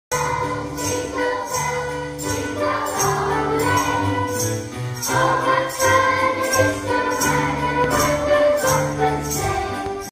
Kerstconcert in Boutersem 2023
Op het kerstconcert van ART-vestiging Boutersem tonen de leerlingen van de muziek- en woordafdelingen hun harde werk van de afgelopen maanden.